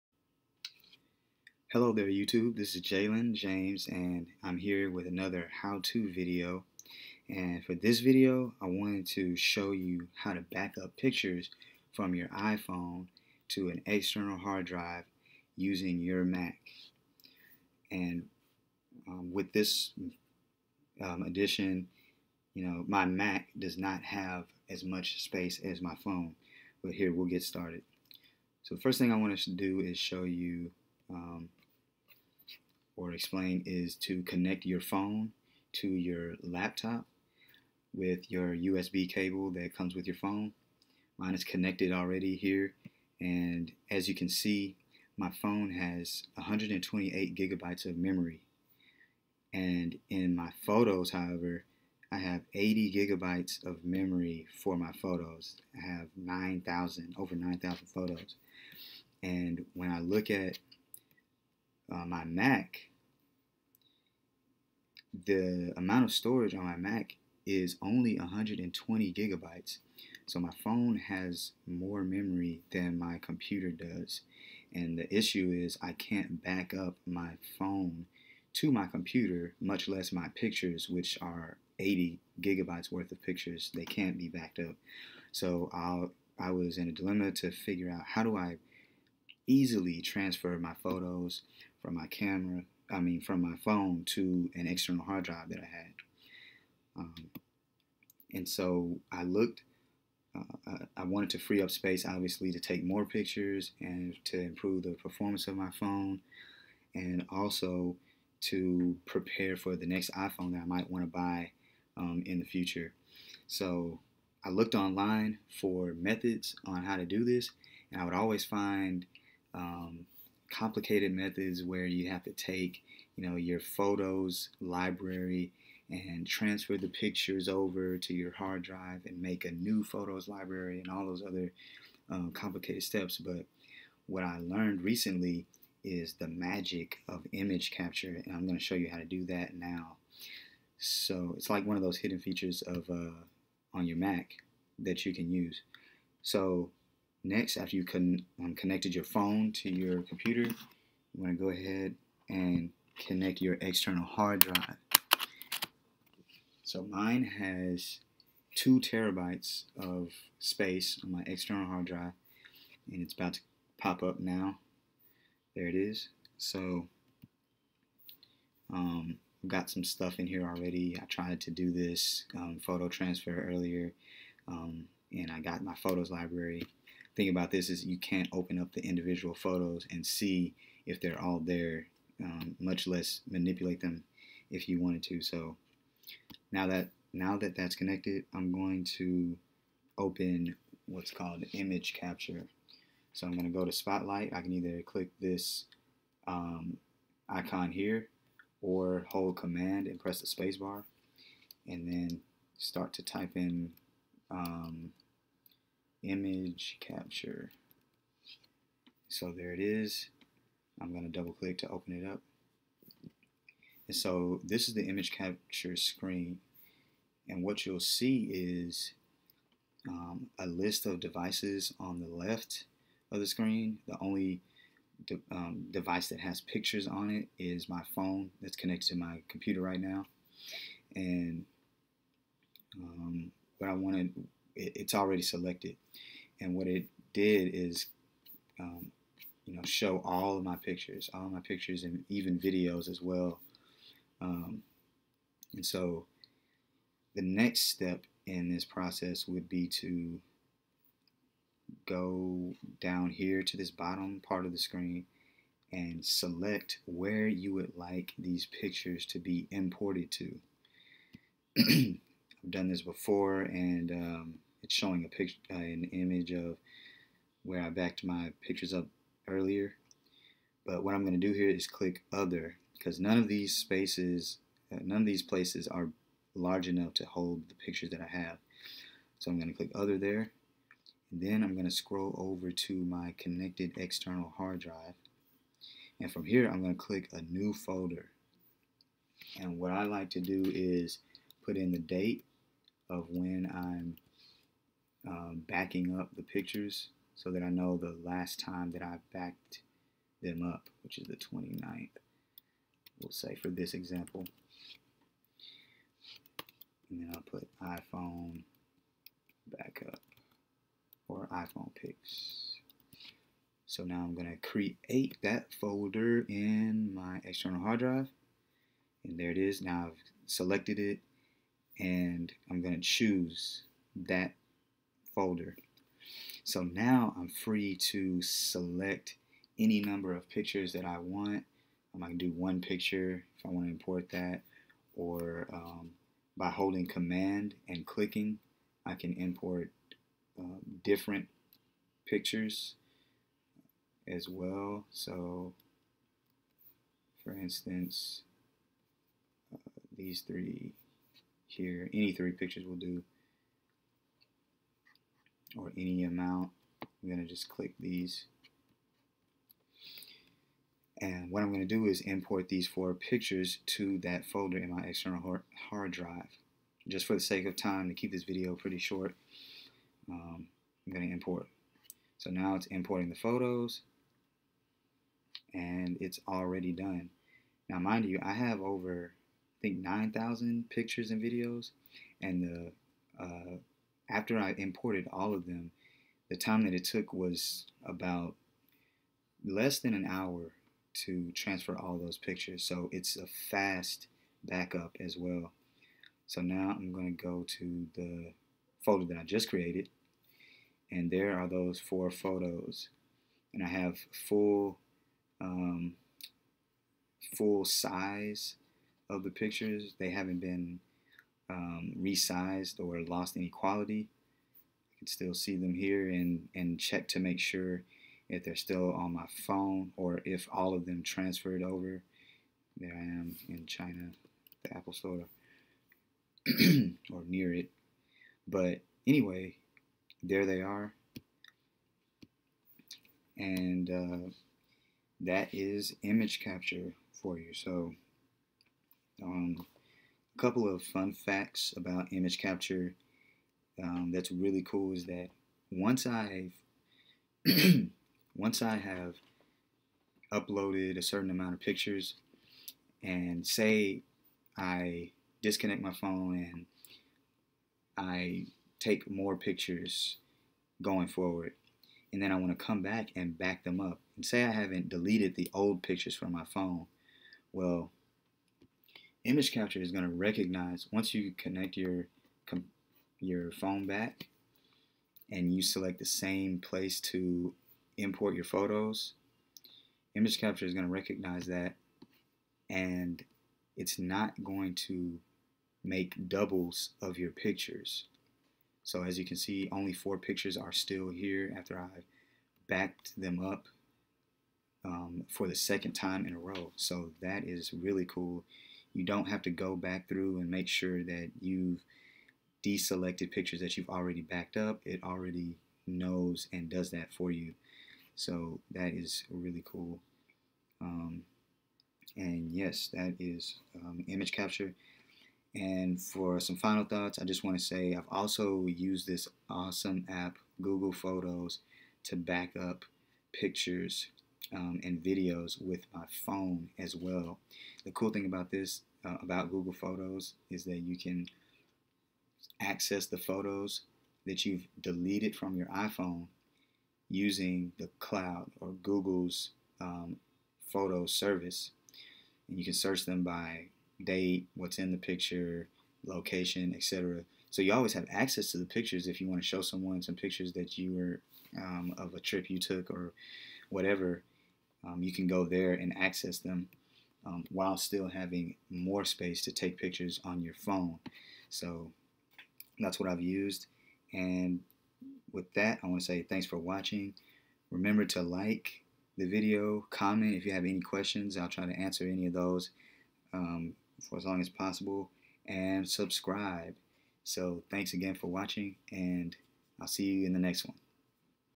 How to Easily Transfer iPhone Pictures to External Hard Drive
Learn how to easily and quickly transfer photos from your iPhone to your external hard drive.

This method works even if your iPhone has more memory than your computer, which was the case for me. Also, no software downloads needed to make the transfer.